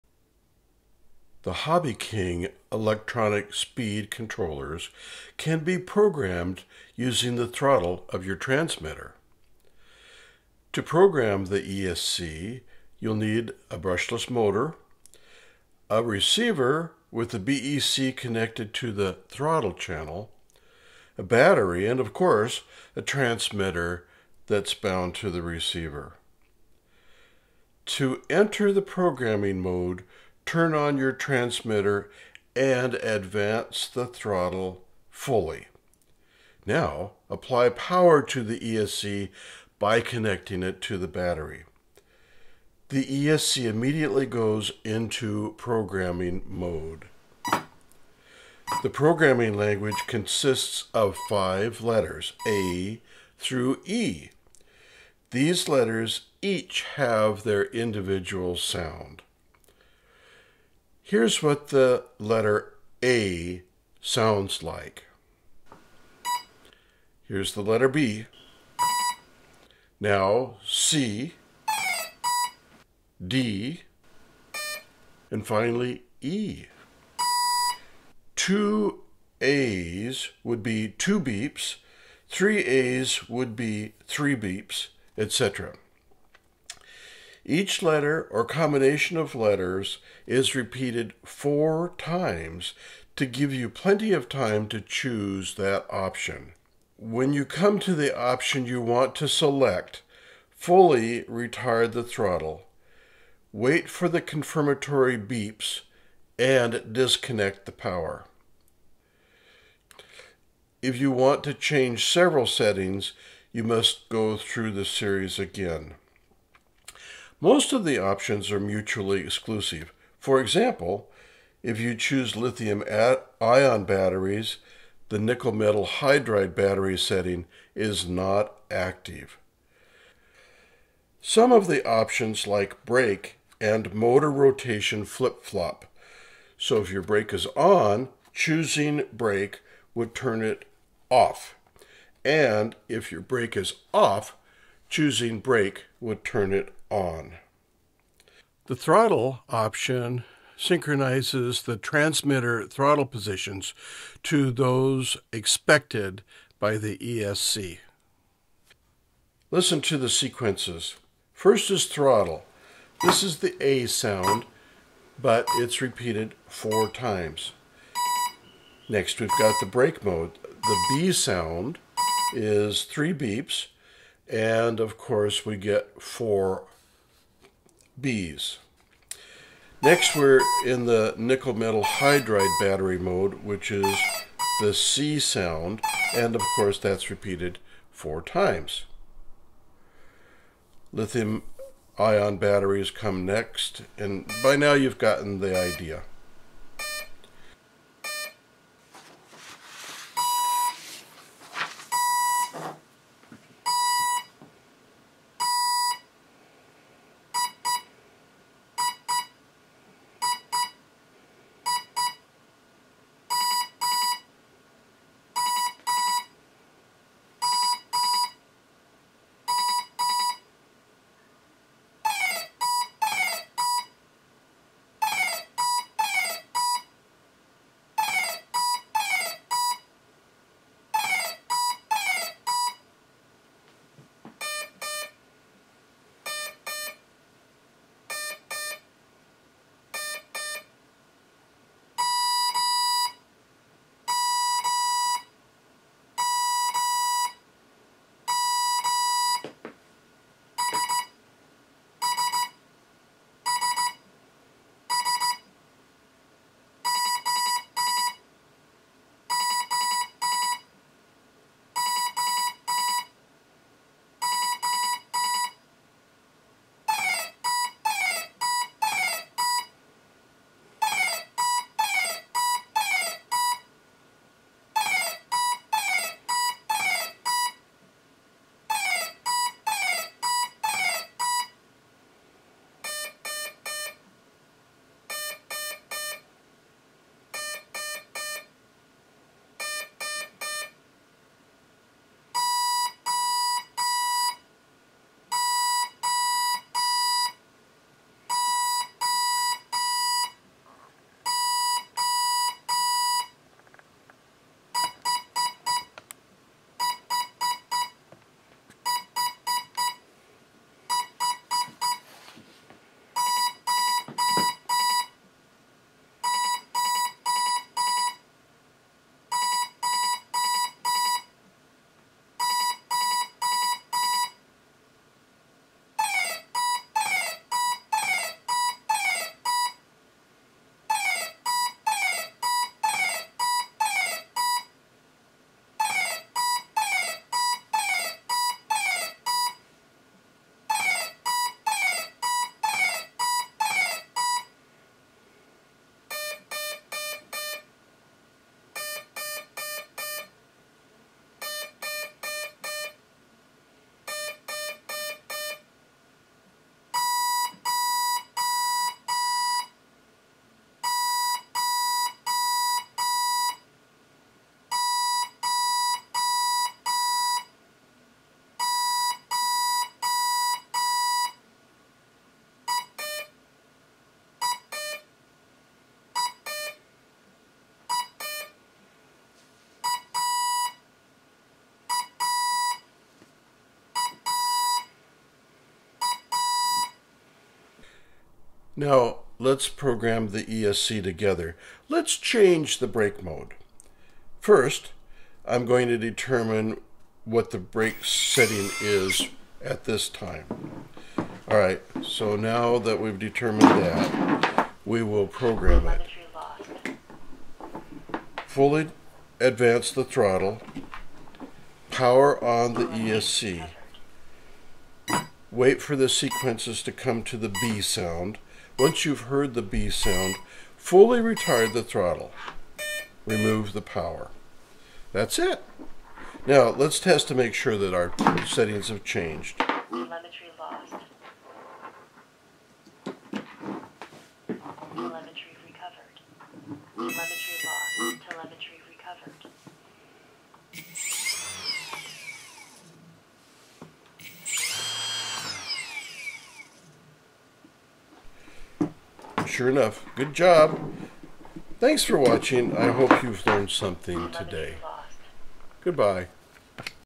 Programming the HobbyKing ESC with the transmitter throttle
How to program the various Hobby King ESC's using the throttle of your transmitter.